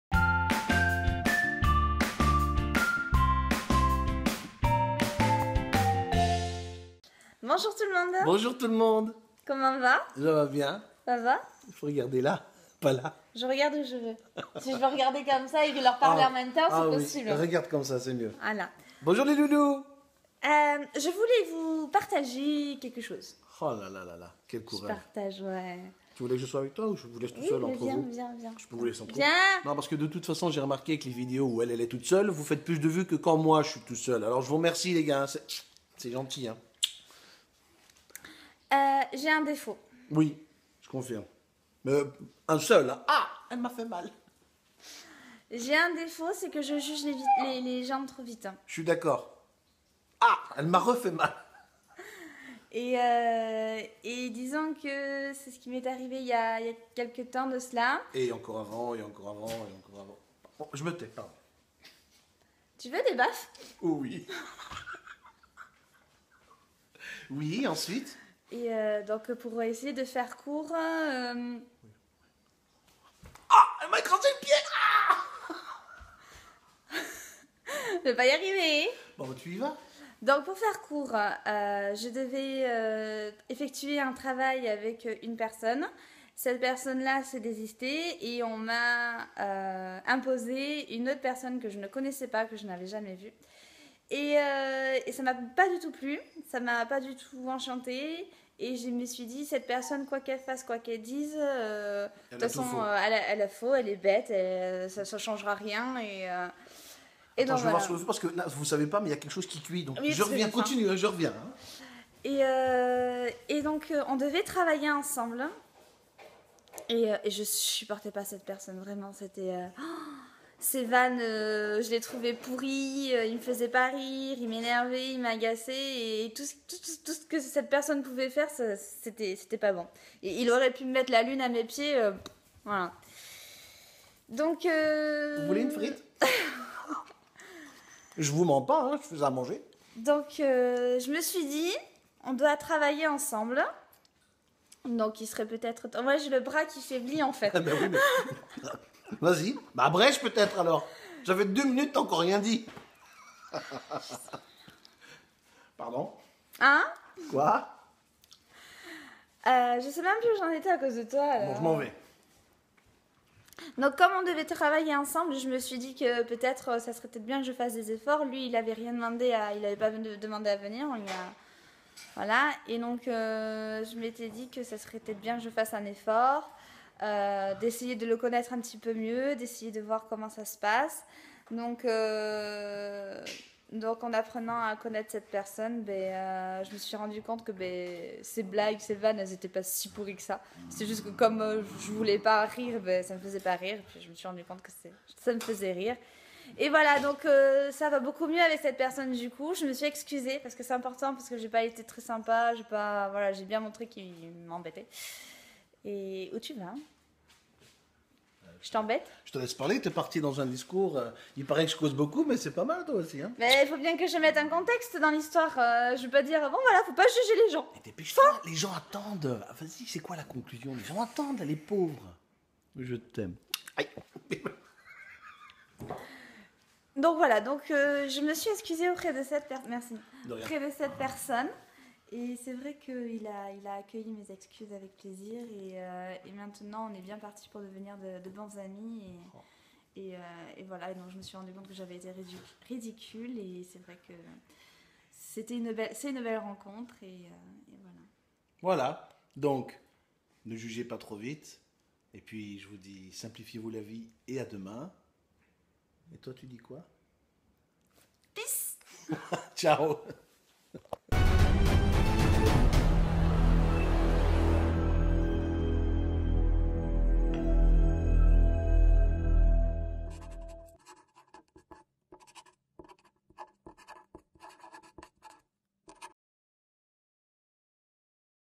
JOUR 150 : Vous arrive-t-il de juger les gens trop vite ? Une réaction optimiste...
Vous arrive-t-il de juger les autres un peu trop vite et puis, avec le temps, vous rendre compte que vous vous êtes trompé ?